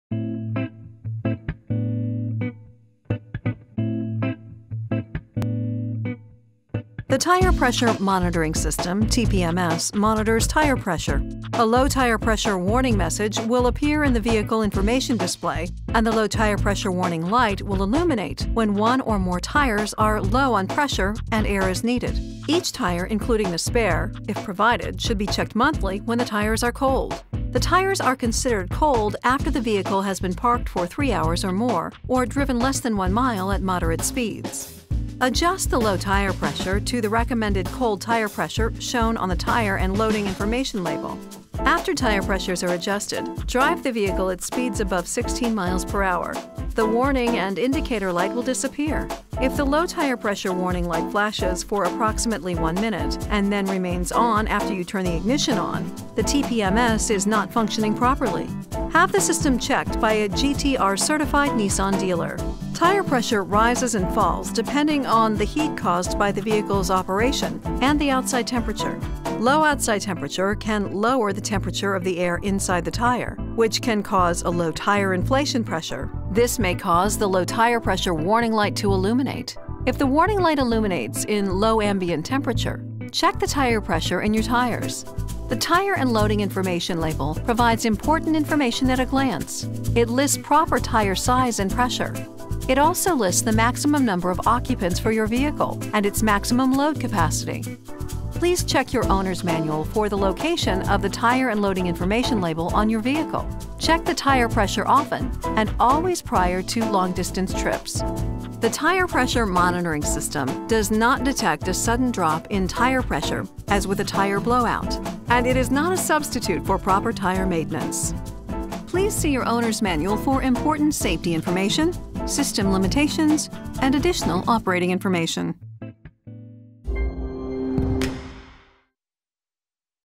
2015 NISSAN GT-R - Tire Pressure Monitoring System (TPMS)
The Tire Pressure Monitoring System (TPMS) monitors tire pressure. A LOW TIRE PRESSURE warning message will appear in the Vehicle Information Display and the low tire pressure warning light will illuminate when one or more tires are low on pressure and air is needed.

Each tire including the spare (if provided) should be checked monthly, when the tires are cold. The tires are considered cold after the vehicle has been parked for 3 hours or more or driven less than 1 mile at moderate speeds.

Adjust the low tire pressure to the recommended COLD tire pressure shown on the Tire and Loading Information label. After tire pressures are adjusted, drive the vehicle at speeds above 16 miles per hour. The warning and indicator light will disappear. If the low tire pressure warning light flashes for approximately one minute and then remains on after you turn the ignition ON, the TPMS is not functioning properly. Have the system checked by a GT-R certified NISSAN dealer.

Tire pressure rises and falls depending on the heat caused by the vehicle's operation and the outside temperature. Low outside temperature can lower the temperature of the air inside the tire, which can cause a low tire inflation pressure.
This may cause the low tire pressure warning light to illuminate. If the warning light illuminates in low ambient temperature, check the tire pressure in your tires.

The Tire and Loading Information label provides important information at a glance.
It lists proper tire size and pressure. It also lists the maximum number of occupants for your vehicle, and its maximum load capacity. Please check your Owner's Manual for the location of the Tire and Loading Information label on your vehicle. Check the tire pressure often and always prior to long distance trips.

The Tire Pressure Monitoring System does NOT detect a sudden drop in tire pressure as with a tire blowout. And it is not a substitute for proper tire maintenance.